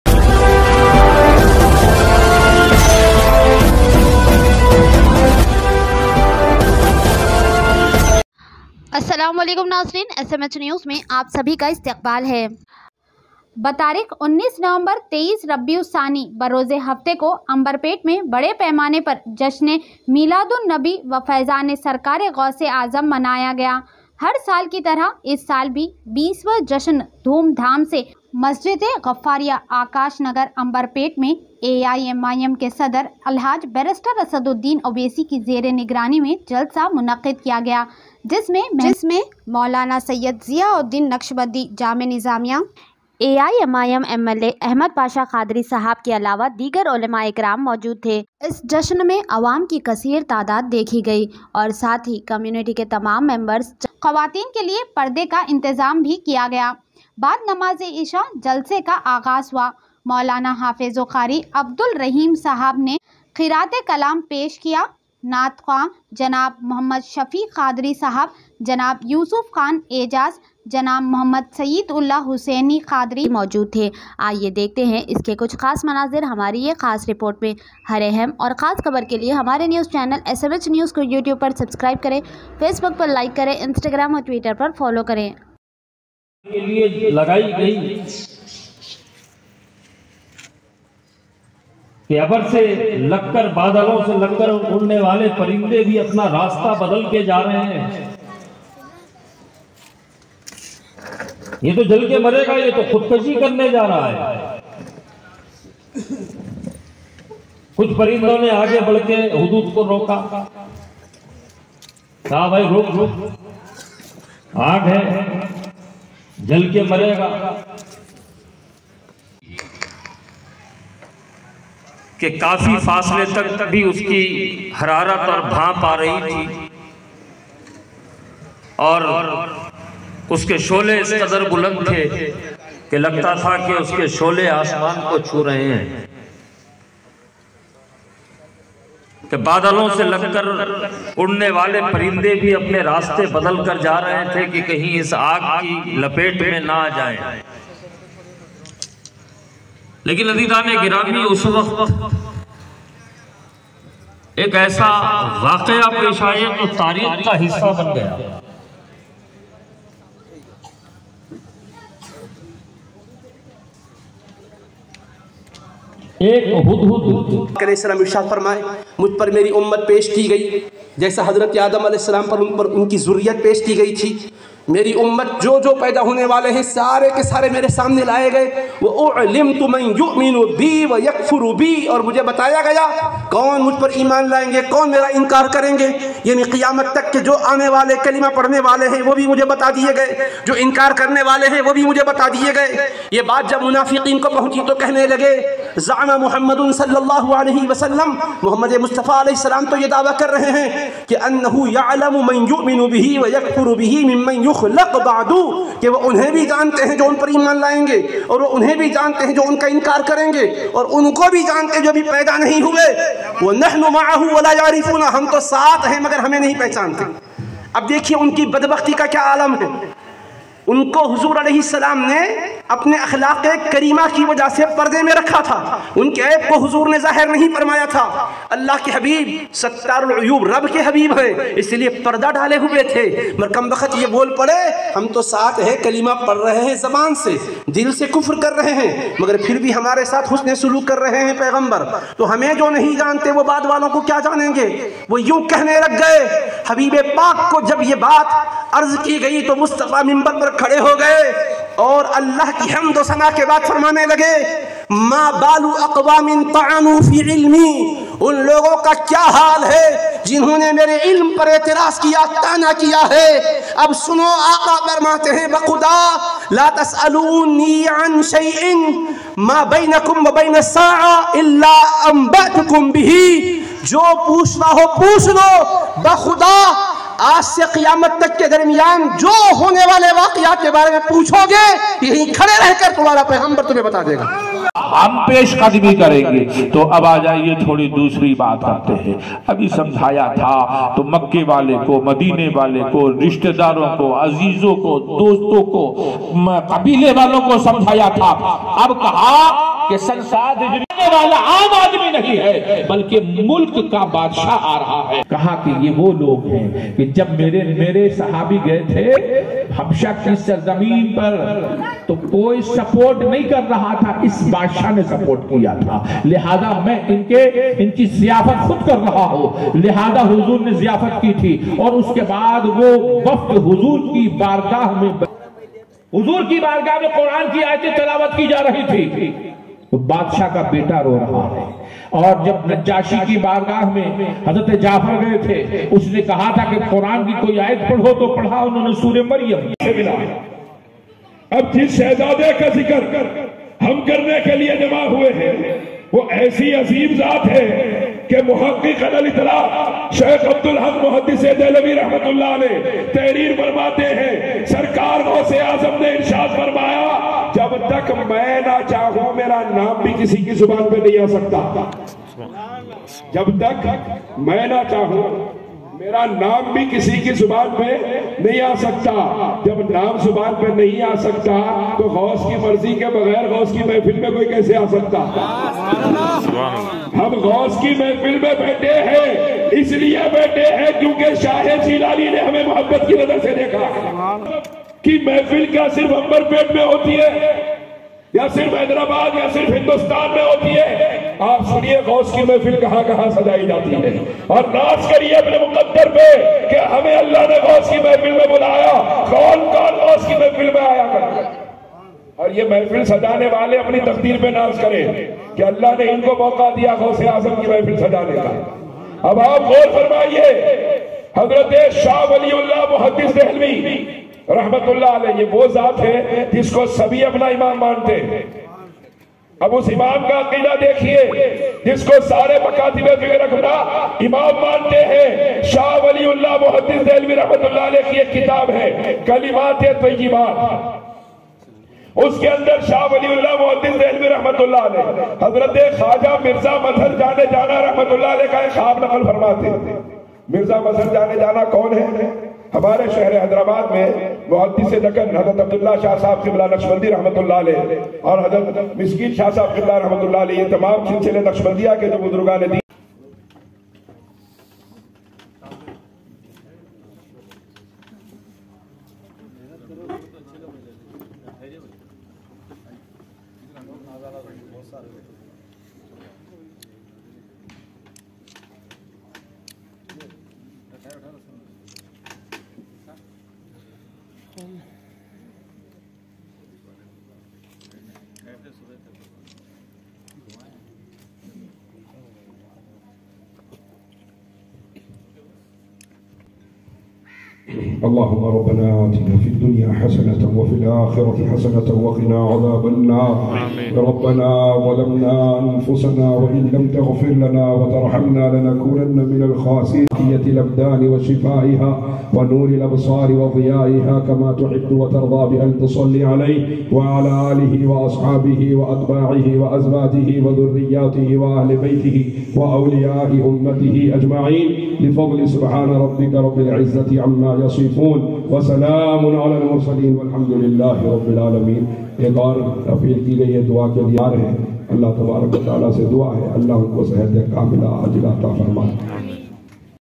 MASJID GAFFARIA AKASH NAGAR AMBERPET ME MILAD UN NABI SAW WOH FAIZAN SARKAR HUZUR GHOUSE AAZAM JALSA
MASJID GAFFARIA
AKASH NAGAR AMBER PET
MEIN 20th JASHN MILAD UN NABI SAW WOH FAIZAN SARKAR HUZUR GHOUSE AAZAM KA JALSA MANAYA GAYA HAI @SMHNEWS_SYEDMISBAH